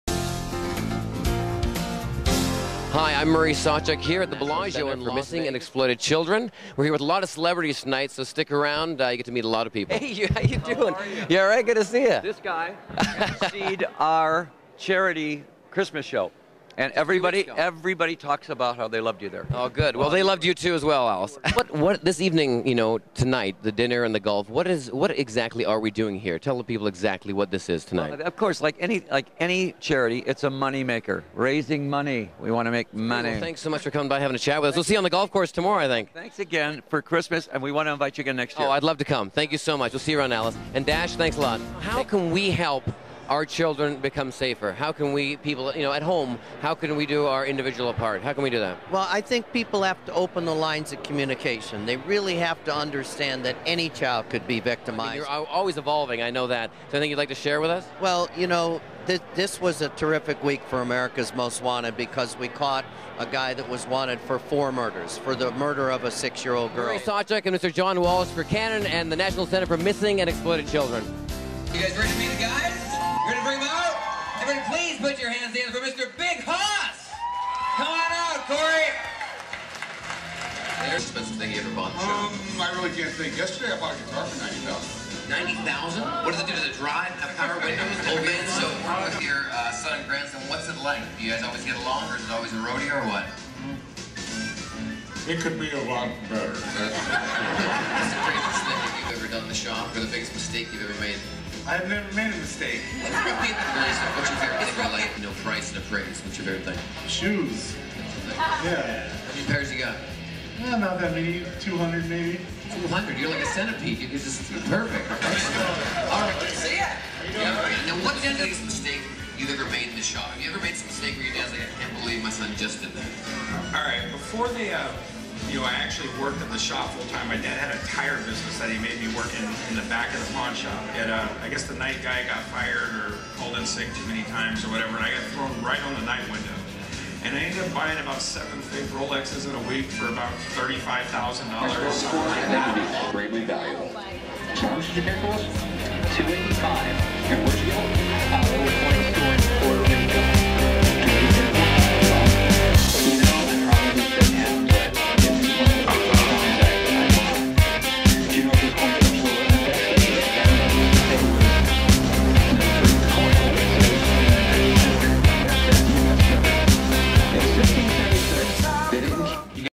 Murray SawChuck Hosts Canon NCMEC & Pawn Stars 'LIVE'!
Murray SawChuck travels worldwide hosting events, shows and charities. Here is a couple events he recently hosted.